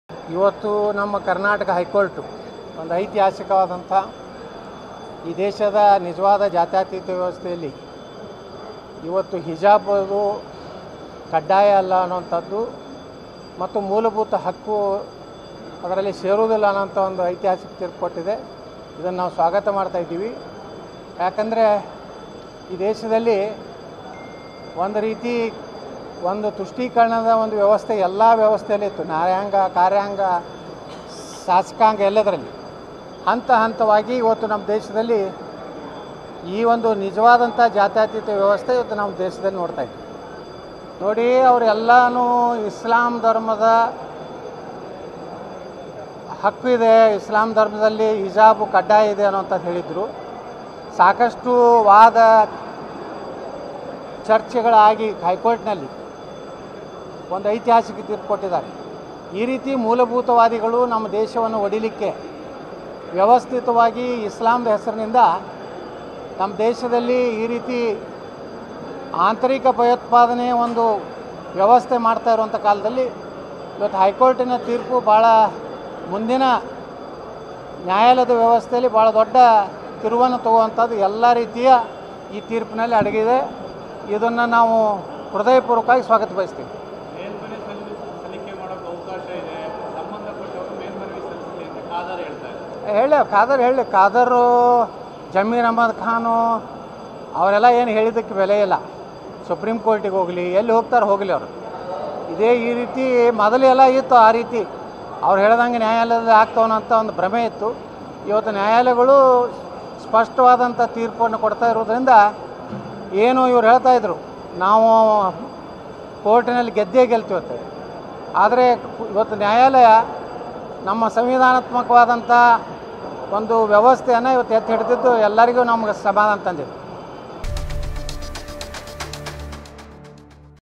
Yathnal : ಜಮೀರ್ ಖಾದರ್ ಹೇಳಿದ್ದಕೆಲ್ಲ ಬೆಲೆ ಇಲ್ಲ
Yathnal : ಜಮೀರ್ ಖಾದರ್ ಹೇಳಿದ್ದಕೆಲ್ಲ ಬೆಲೆ ಇಲ್ಲ ಹಿಜಾಬ್ ವಿವಾದದ ಬಗ್ಗೆ ಕೋರ್ಟ್ ನೀಡಿದ ತೀರ್ಪನ್ನ ಸ್ವಾಗತಿಸಿದ ಶಾಸಕ ಯತ್ನಾಳ್|